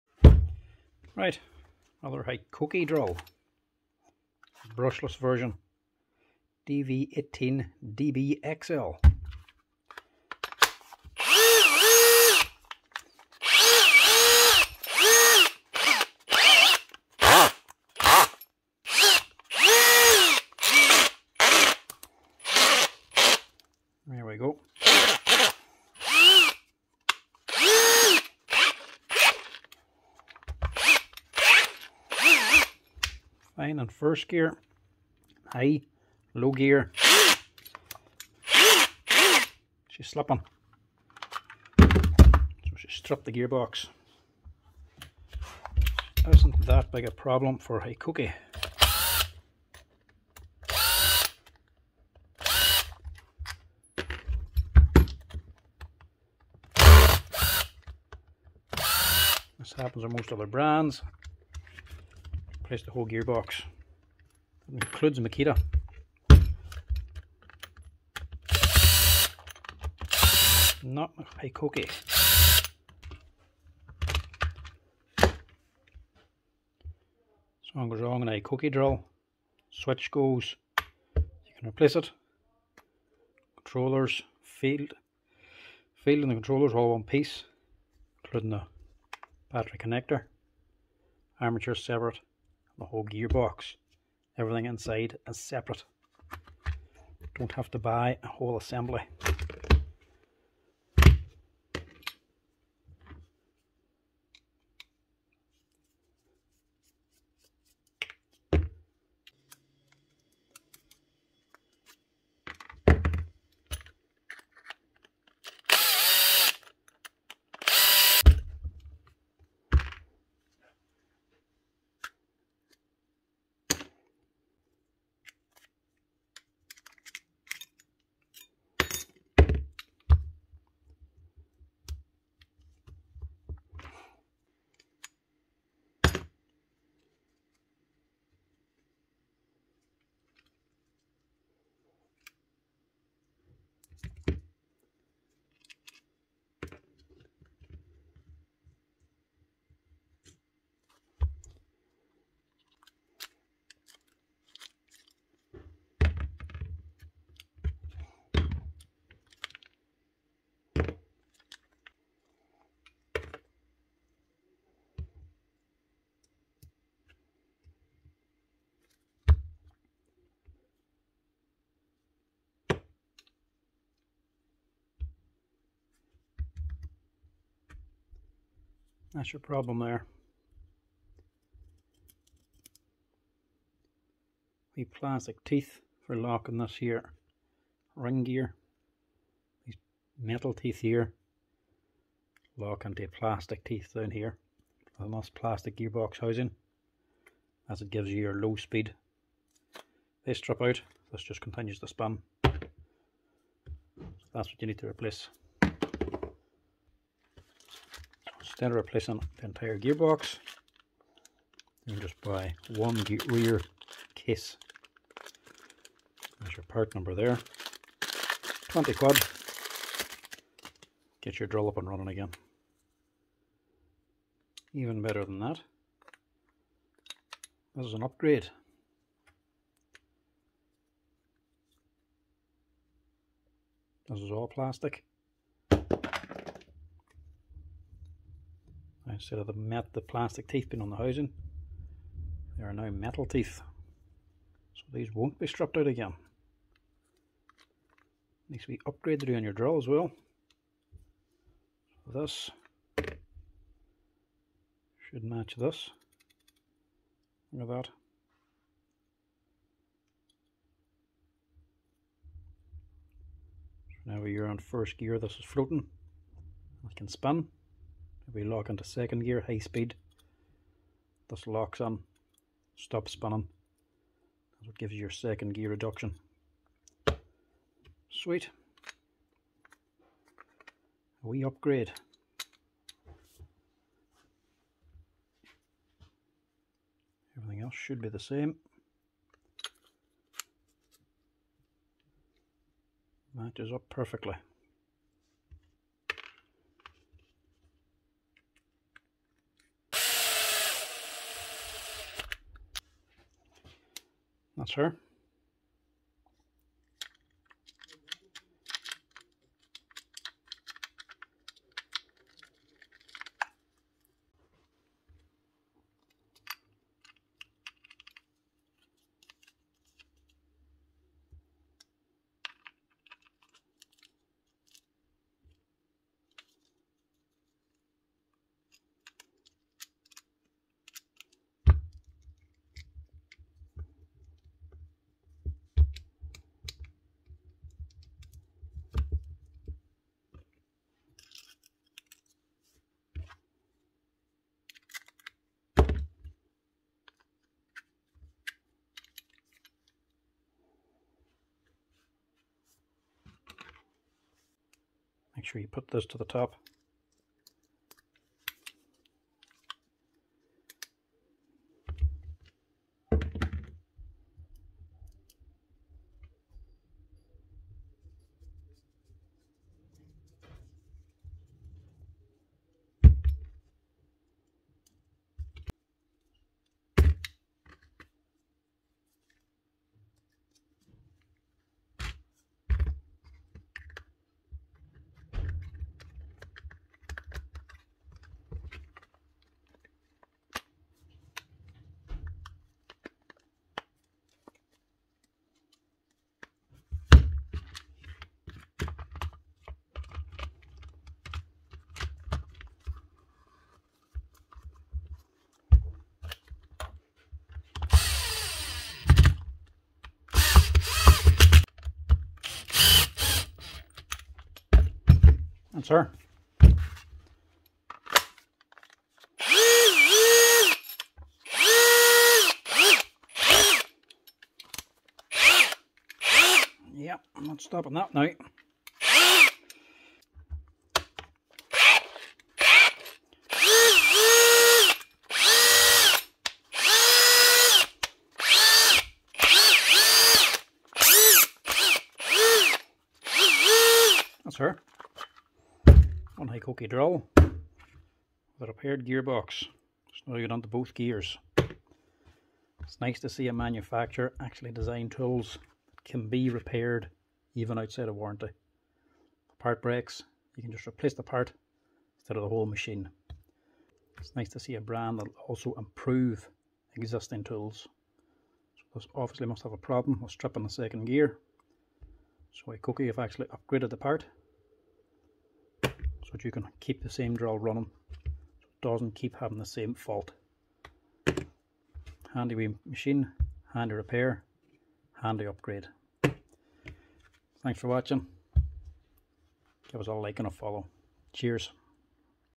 Hikoki brushless drill slipping in low gear repair. Stripped gearbox housing.
This is a common fault for any brand of drill. it's always the low gear that eventually starts ti slip.
This is one the main reasons I like Hikoki tools. They don't sell part assemblies. Instead you can buy the part you need, instead of an inter gearbox. Not many brands offer this range of spare parts.

Not only can you buy this gear box housing for 20 quid, it is also an upgraded part.
Hikoki seen this part failing and upgraded it yo a metal part instead, so no more stripped gears.

Really starting to like hikoki tools. I just hope the part availability improves. As time is the biggiet drawback now waiting for parts to come in.